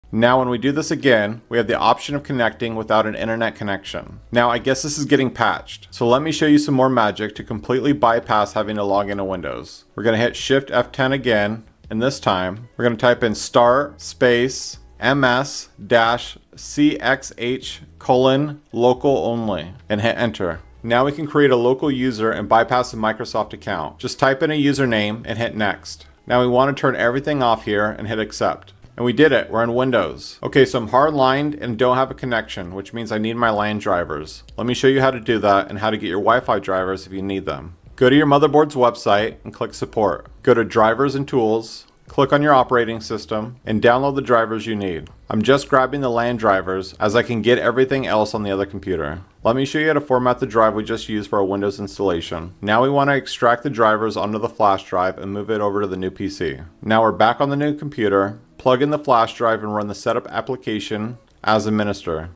How To Skip Microsoft Login On Windows In 2025!
This video shows you how to bypass the Microsoft account requirement during Windows setup and create a local account. The video also shows how to install Windows 11 without microsoft account. The steps include using commands to bypassnro and set up a local user account instead.